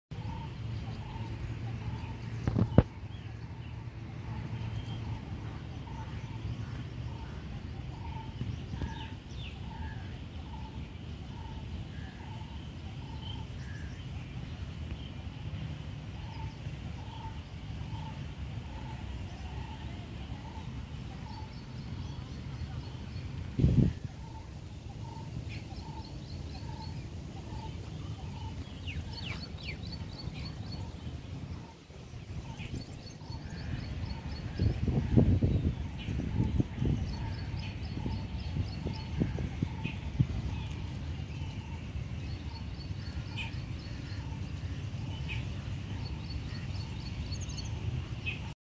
Yellow And Pink Rose : Beautiful Creation Of God.
Yellow roses are the color of friendship. They represent joy and caring, and the bright color can also be seen as a reference to the sun and the warmth that it provides. The yellow rose has become the perfect flower choice for gifting to a close friend.
They are also a good option for brightening up another person’s day, as they radiate positive energy and happiness.

A deep pink rose tends to symbolize gratitude and appreciation, making them another great choice for saying, “Thank you!”